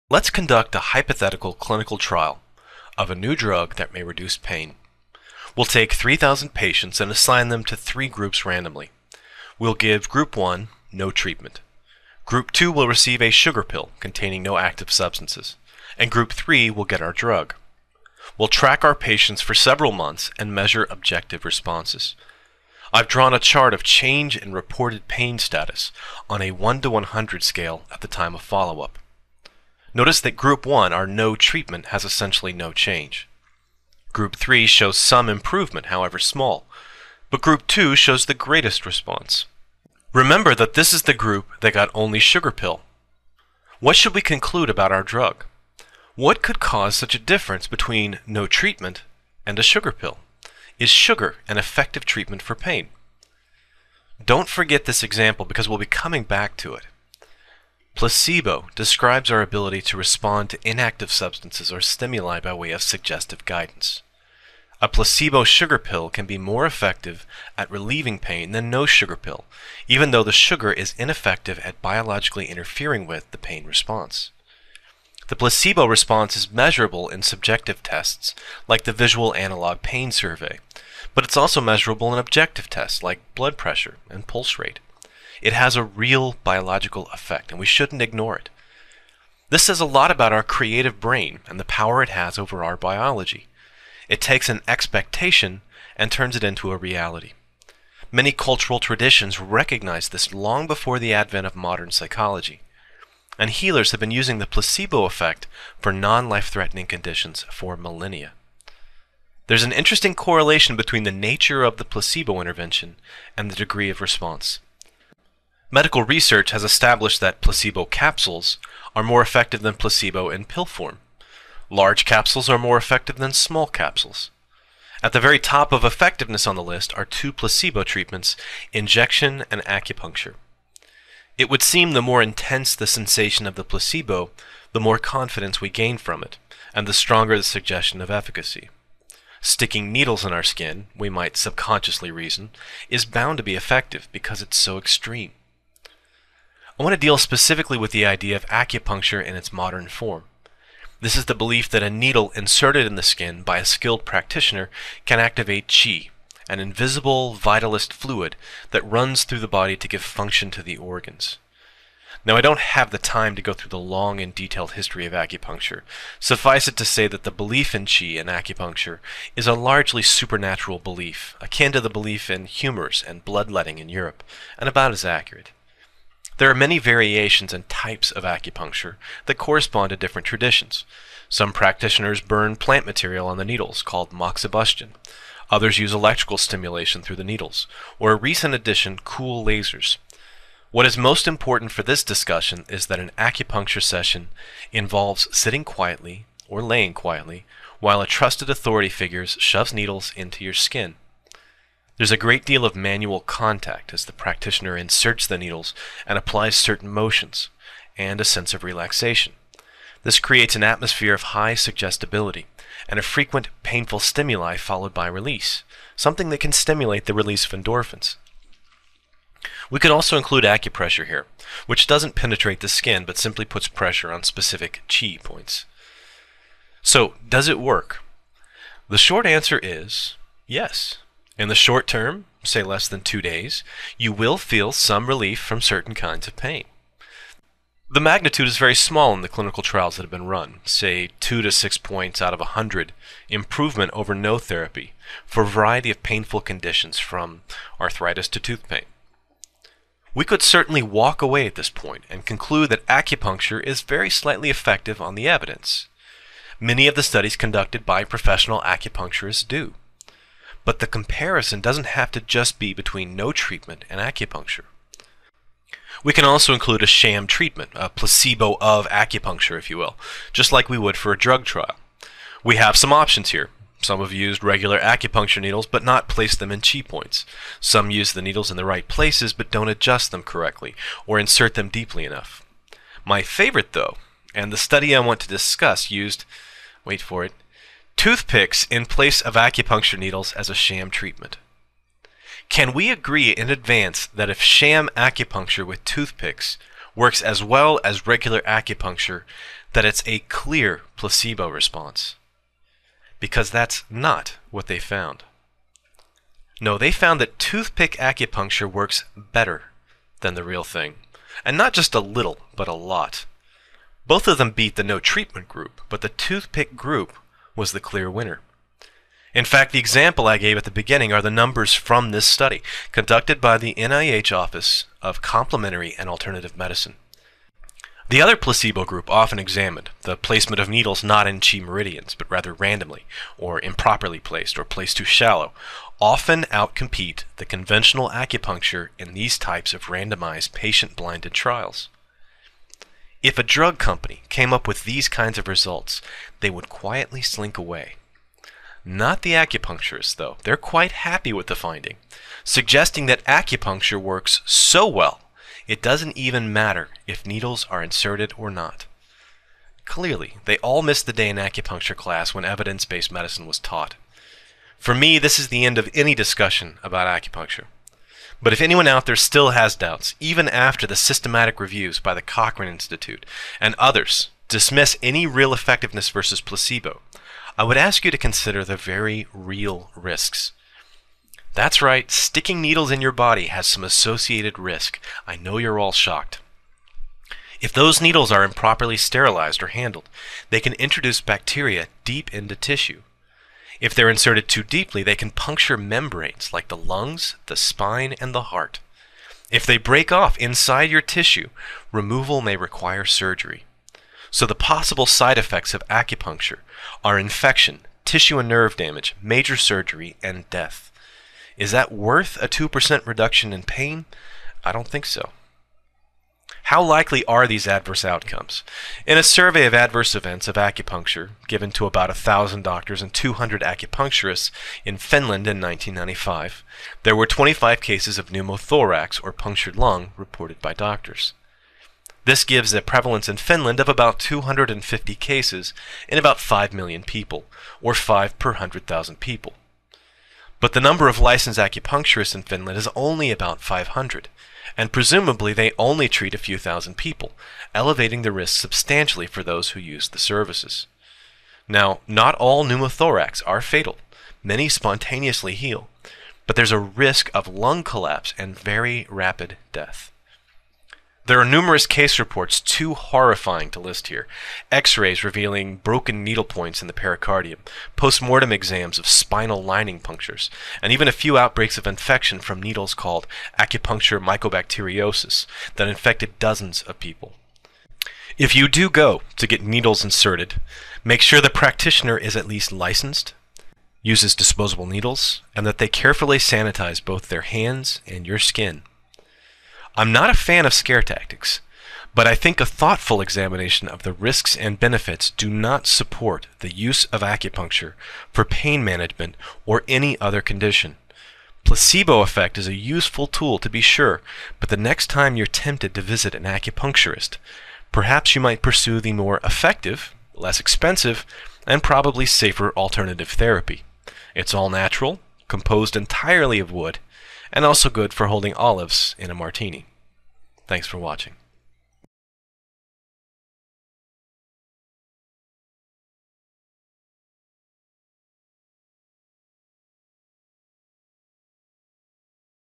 Acupuncture
Is acupuncture effective at treating pain or disorders?  I'll give the scientific answer to that question, and explain why I don't recommend acupuncture for any condition.

A few notes, and some references:
1.  Toothpick acupuncture does NOT involve inserting the toothpick in the skin.  It was merely placed against the skin in a needle guide-tube to give a sharp sensation, then removed.

2.  In the USA, acupuncturists are required to meet sterile standards, use one-use-only needles or autoclaved (sterilized) needles, and must be licensed to practice.

3.  I'm not really addressing the acupressure and non-insertive traditions of some Japanese acupuncture forms.  Certainly these are similar to non-insertive sham treatments, but it should be noted that the placement of fingers or toothpicks is not important for the effect.

References:
1.  BMJ 2009;338:a3115
Acupuncture treatment for pain: systematic review of randomised clinical trials with acupuncture, placebo acupuncture, and no acupuncture groups

2.  Arch Intern Med. 2009;169(9):858-866
A Randomized Trial Comparing Acupuncture, Simulated Acupuncture, and Usual Care for Chronic Low Back Pain

3.  Clin Med 2006;6:381--6
Systematic review of systematic reviews of acupuncture published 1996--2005

4.  Lancet.  1995 Jun 17;345(8964):1576.
Adverse effects of acupuncture.